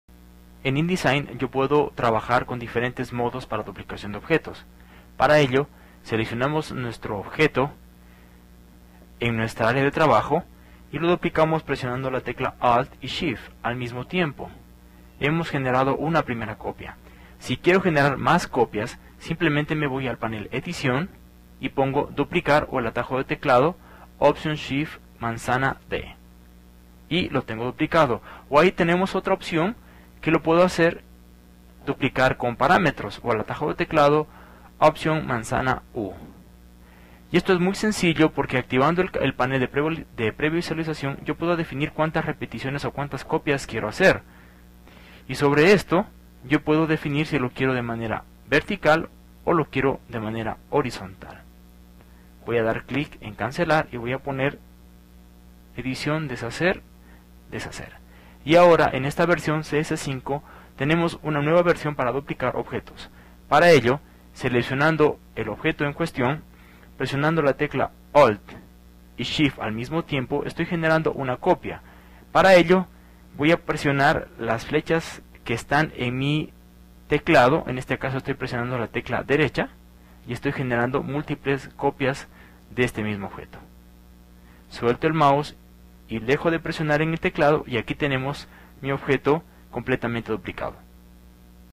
✅ COMO DUPLICAR OBJETOS Adobe Indesign | TUTORIAL
Desde la versión CS5 tienes varias opciones para Duplicar objetos de una manera rápida y sencilla. Aquí te explico todas las posibilidades.